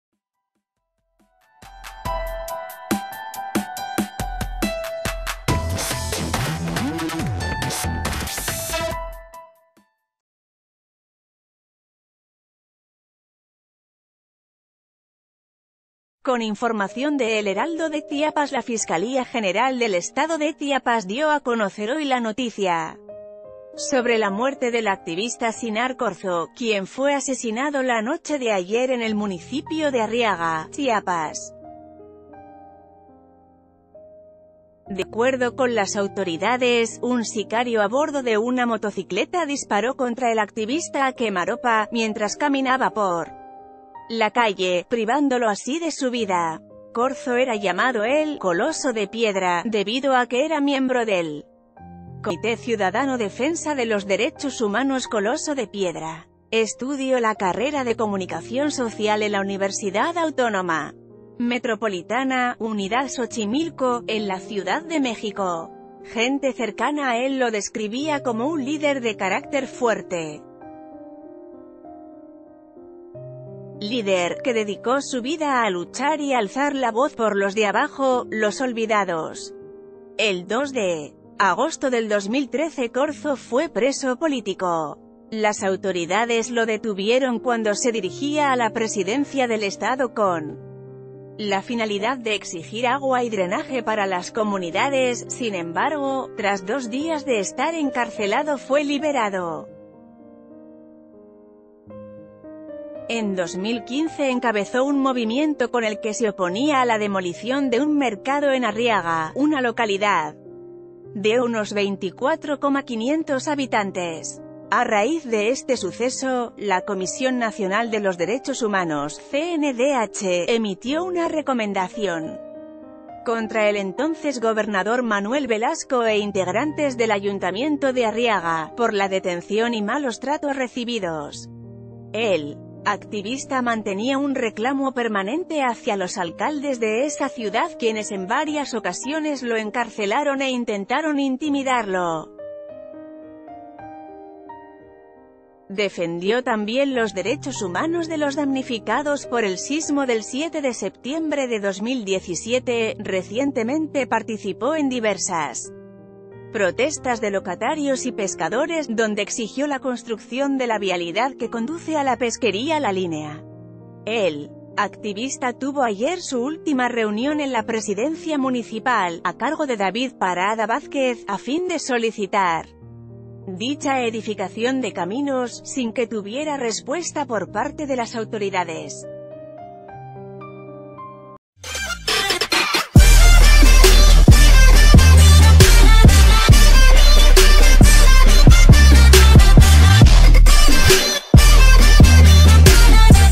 ¿Quién era Sinar Corzo, el activista asesinado en Chiapas?
Con información de El Heraldo de Chiapas La Fiscalía General del Estado de Chiapas dio a conocer hoy la noticia sobre la muerte del activista Sinar Corzo, quien fue asesinado la noche de ayer en el municipio de Arriaga, Chiapas. De acuerdo con las autoridades, un sicario abordo de una motocicleta di...